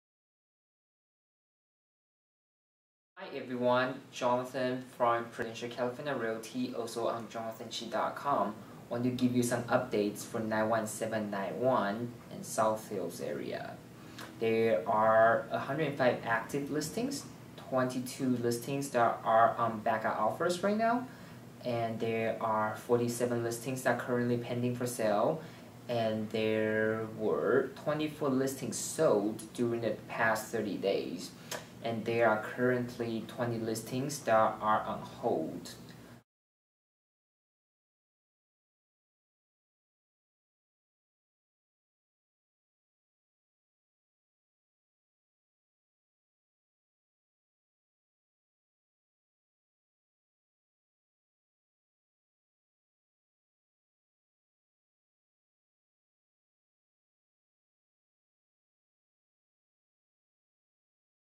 06-06-11 91791 West Covina/South Hills Real Estate Activity
Total Active Listings:  105 Listings

Single Family Home:  90
Condo:  10
Townhouse:  5

Backup Offers:  22 Listings

Single Family Home:  17
Condo:  3
Townhouse:  2

Pending for Sale:  47 Listings

Single Family Home:  38
Condo:  7
Townhouse:  2

Closed During the Past 30 Days:  24 Listings

Single Family Home:  21
Condo:  1
Townhouse:  2

Hold Do Not Show:  20 Total

Information Courtesy of CRMLS.